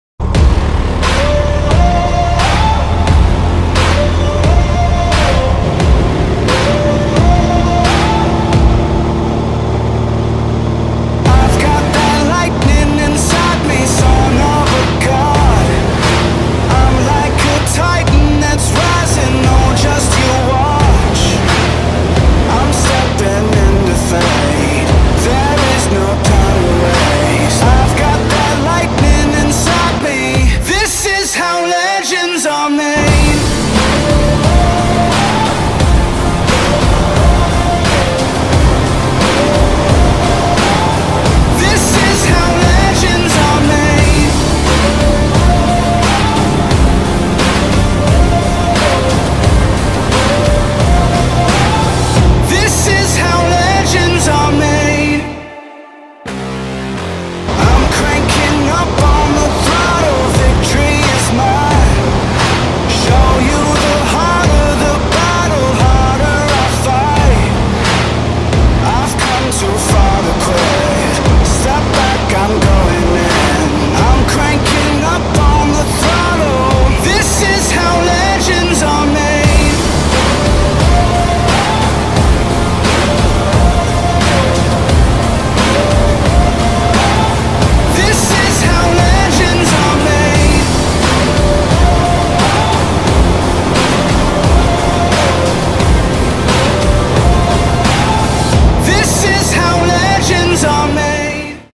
Vans RV-4 Sunset Flight Aerobatics
First flight after picking up the new plane. Light aerobatics and low level in the RV-4.

Vans, RV-4, RV-8, Aerobatics, Flying, Vans RV4, Aviation, Vans RV 4